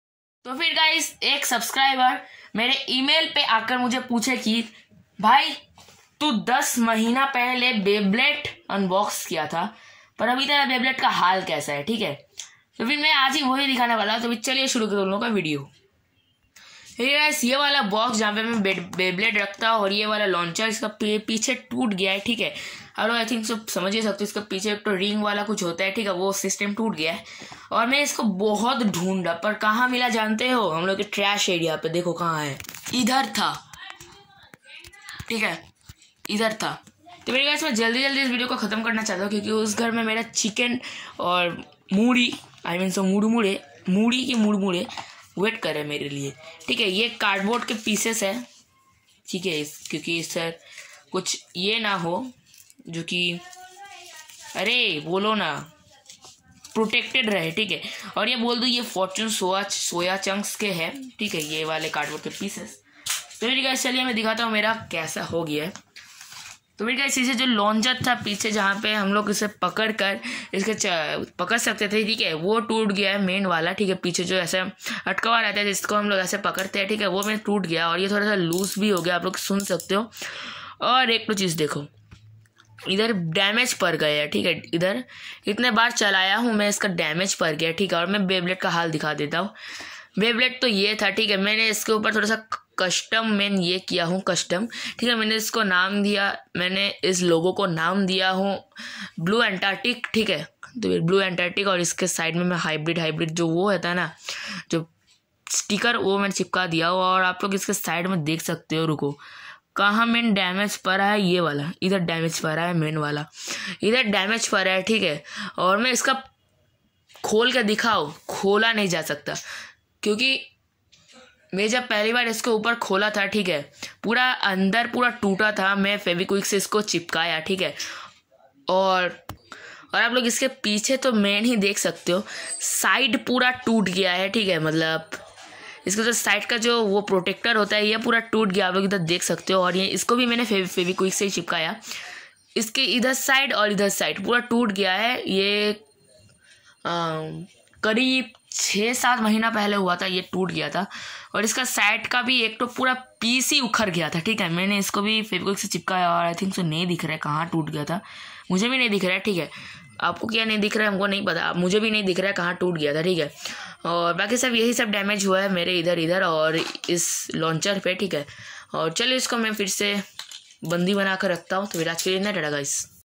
How my Dark Bull Beyblade looks like, 2 years later a beyblade video.
A beyblade reviewer channel.